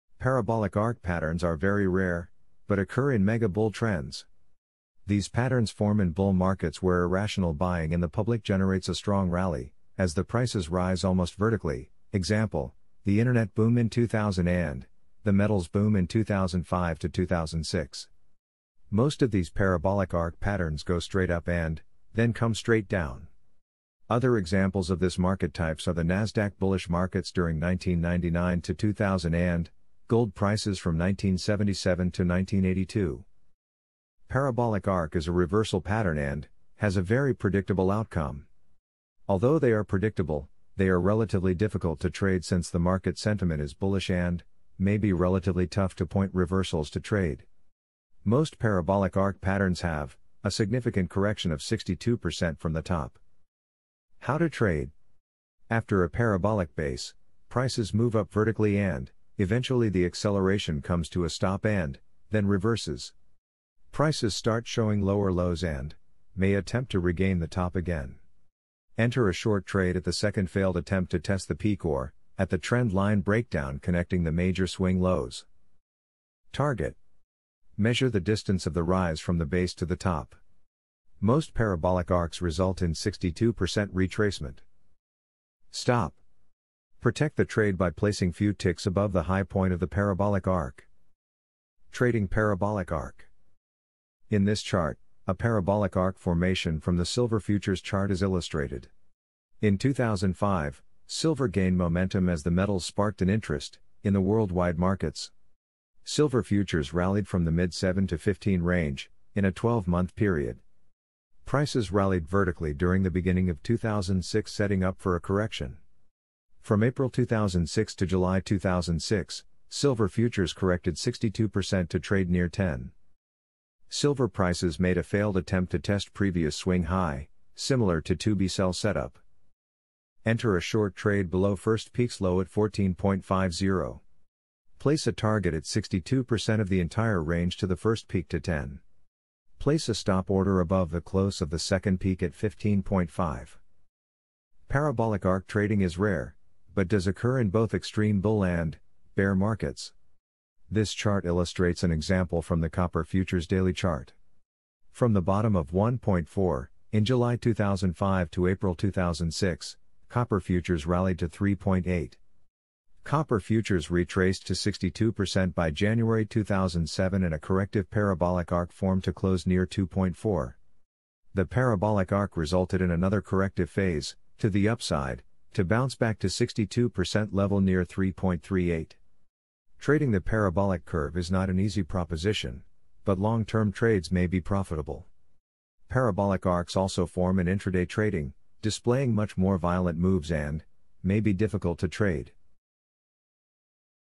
Parabolic Arc Pattern
Parabolic Arc patterns are very rare, but occur in mega bull trends. These patterns form in bull markets where irrational buying in the public generates a strong rally as the prices rise almost vertically, e.g. the Internet Boom in 2000 and the Metals boom in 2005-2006. Most of these "Parabolic Arc" patterns go straight up and then come straight down. Other examples of this market types are the NASDAQ bullish markets during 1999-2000 and Gold prices from 1977 to 1982.